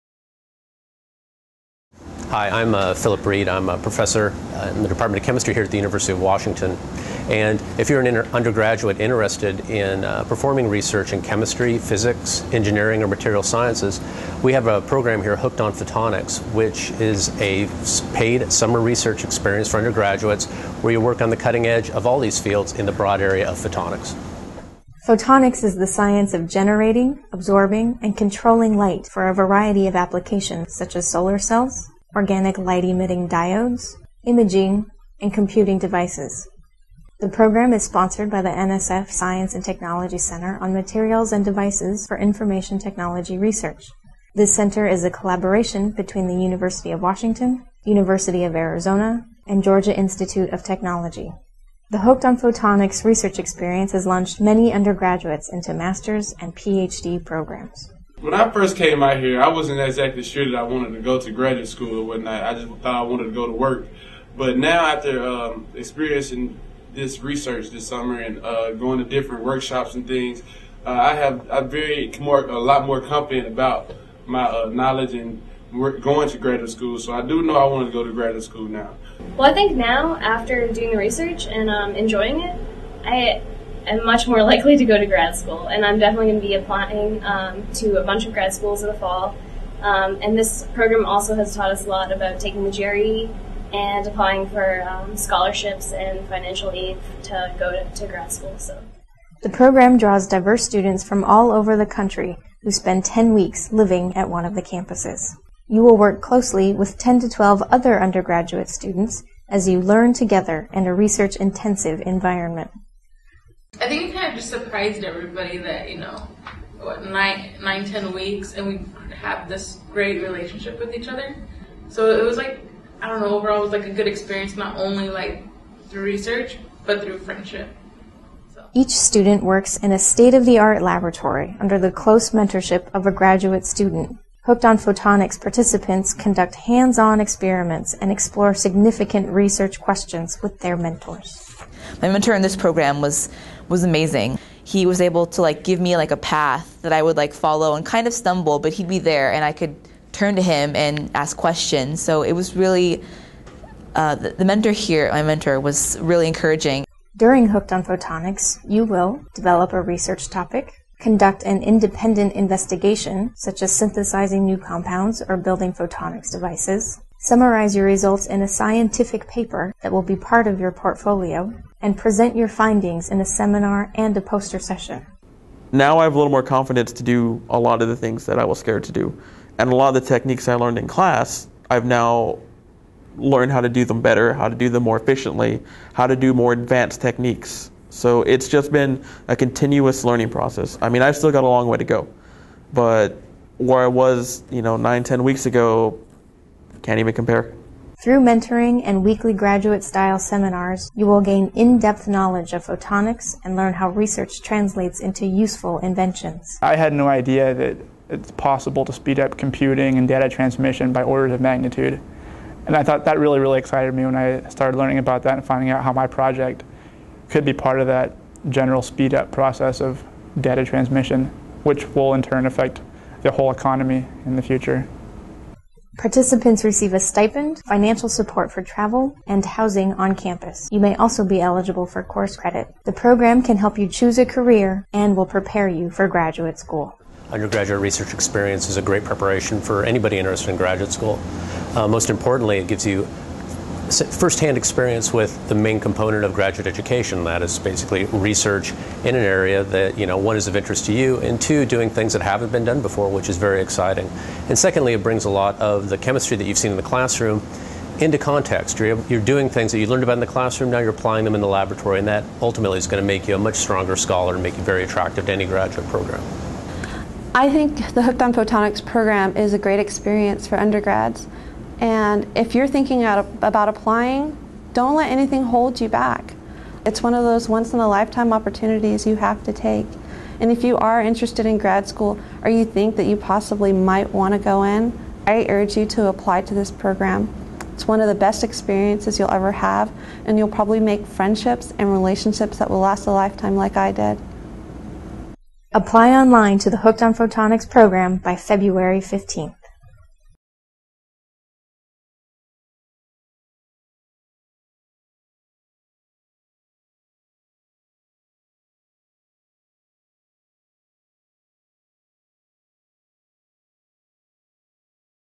Hooked On Photonics Summer REU Program
Hooked on Photonics is a paid summer internship that gives undergraduate students valuable research experience in chemistry, physics, engineering, and materials science. Apply online by February 15th!